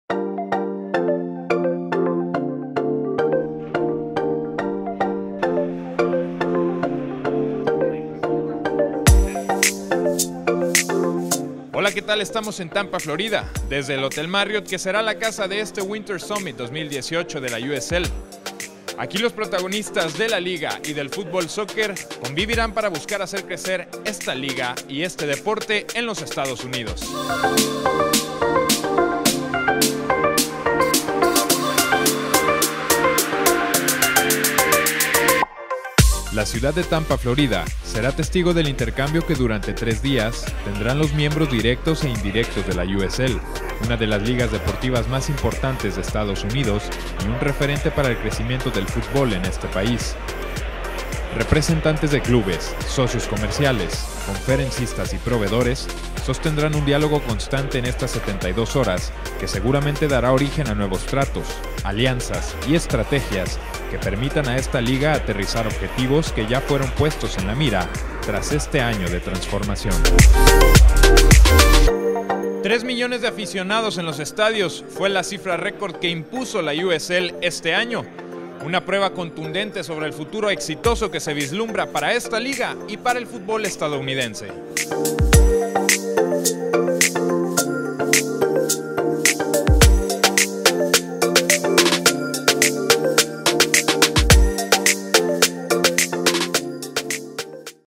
USL Winter Summit 2018
Con más de 100 organizaciones participando, arrancó el Winter Summit de la USL en Tampa, Florida. // With more than 100 organizations participating, the USL Winter Summit has begun in Tampa, Florida.